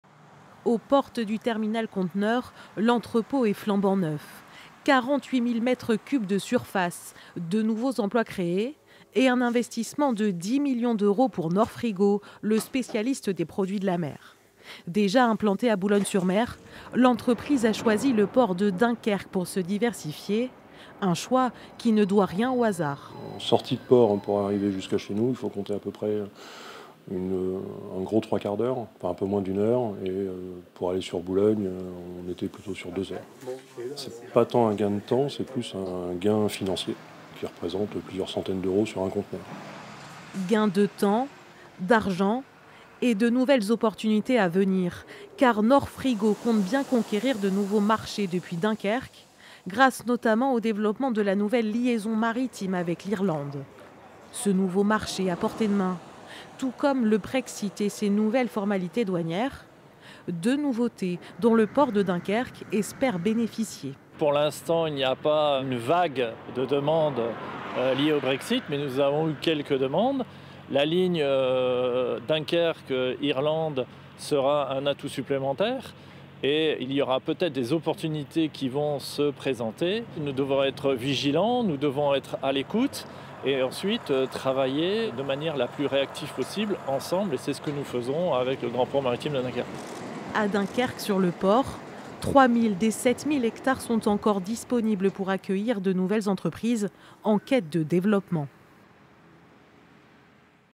48.000 mètres cube de surface, nouveaux emplois, investiment de 10 millions d'euros pour Nordfrigo.
Aux portes du terminal conteneur, l'entrepot est flambant neuf. 48.000 mètres cube de surface, de nouveaux emplois créés et un investiment de 10 millions d'euros pour Nordfrigo, le spécialiste des produits de la mer. Déja implanté à Boulogne-sur-mer, l'entreprise à choisi le port de Dunkerque pour se diversifier. Un choix qui ne doit rien au hasard.
Un reportage de Lila Haffaf, Flavien Bellouti, Marc Graff